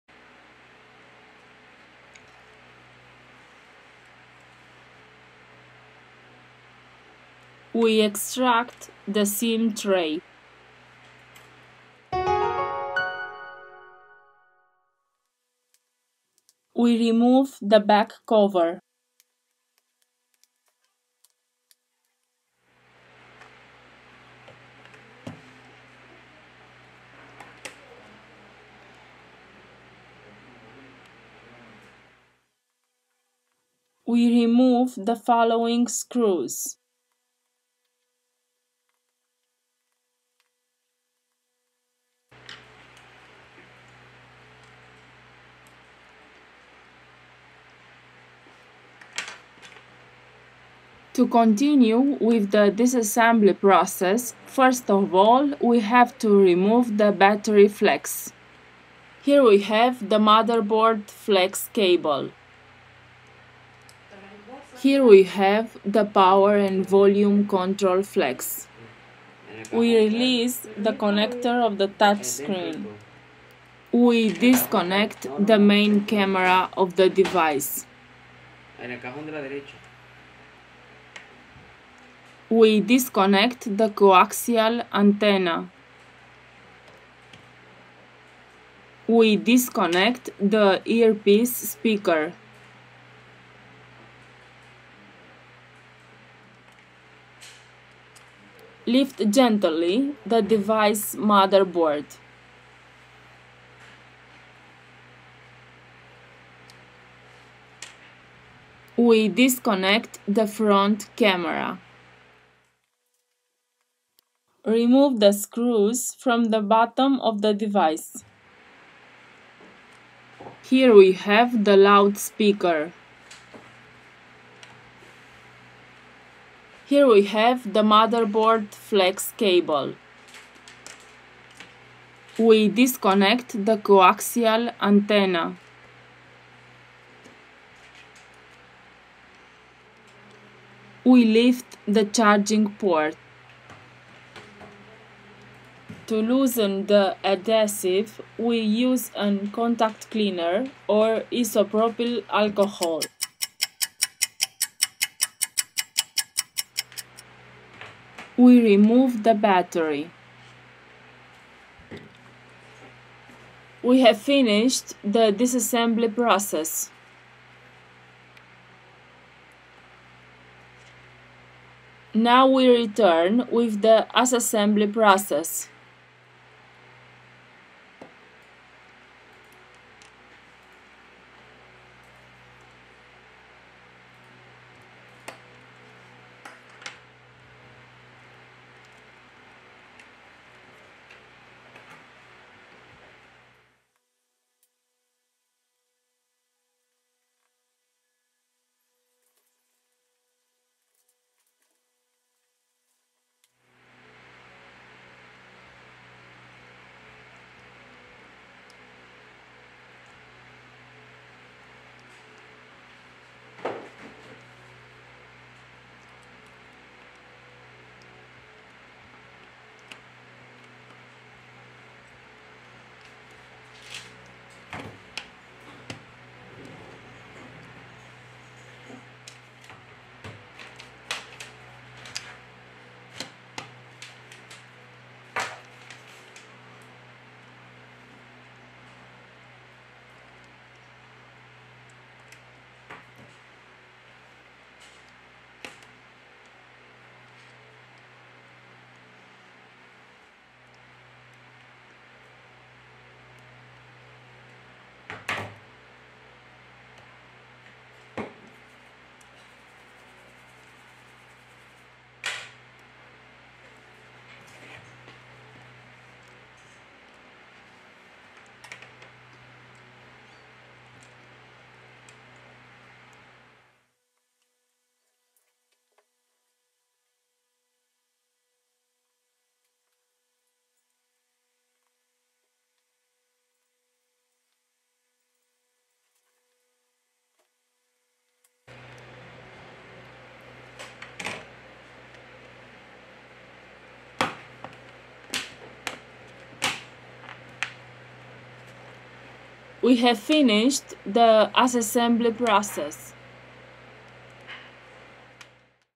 Alcatel Idol 3 Teardown
We had a look inside of the Alcatel Idol 3  and we present the complete process of  disassembly & reassemble.
✅ Phone Case
✅ Hydrogel Film

If you have any question about how to disassembly or any question about any repair of Alcatel Idol 3  please leave your comment above and we ll be glad to answer it.